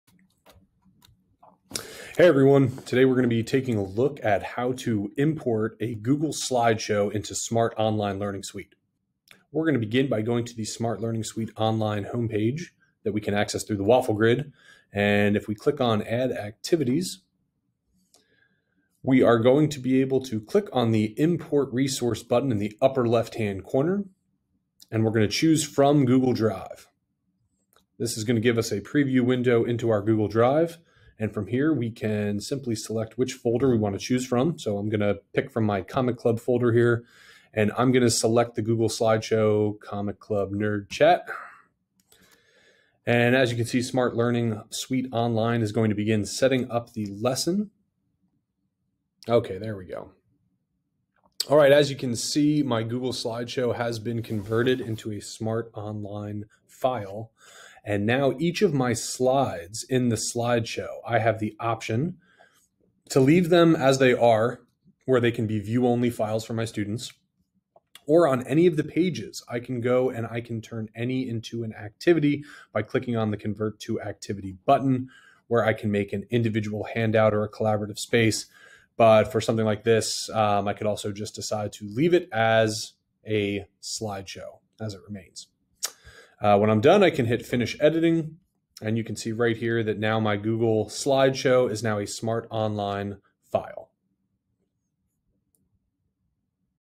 SMART Online - Google Slide Import
This short video will show you how to import a Google Slideshow into SMART Online and how to convert slides into individual or collaborative activities.